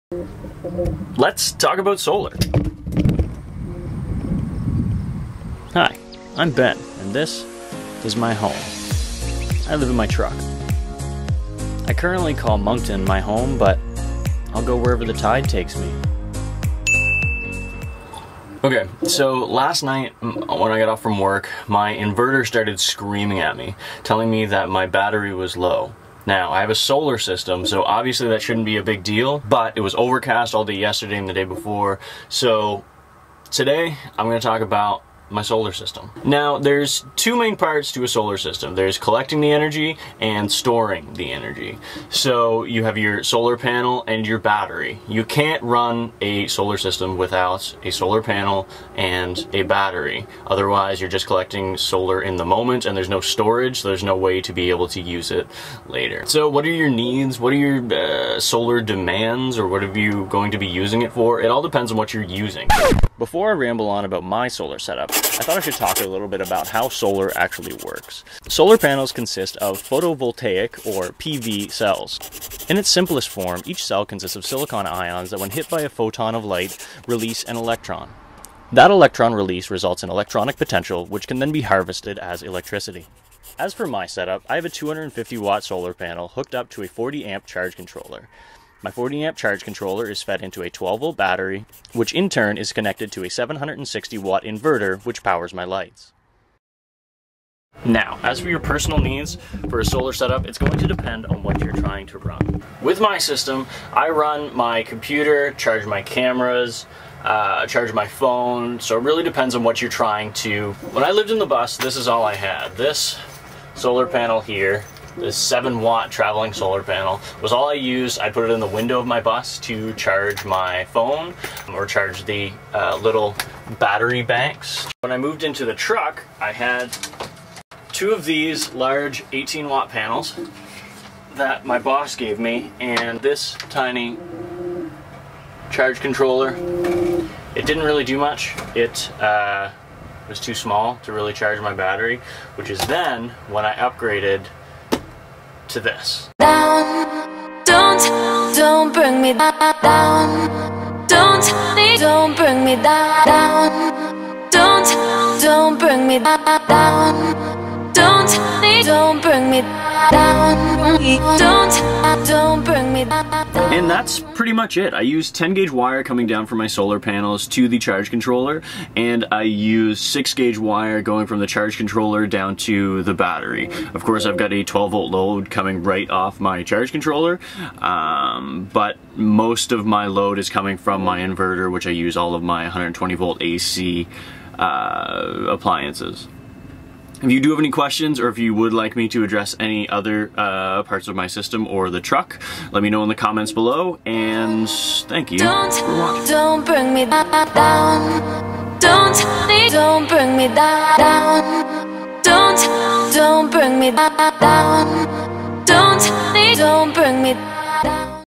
My Journey to Total Solar Dependence!
I started with a 7W USB travellers panel to charge my phone... Now I run a full 250W panel and 12V battery!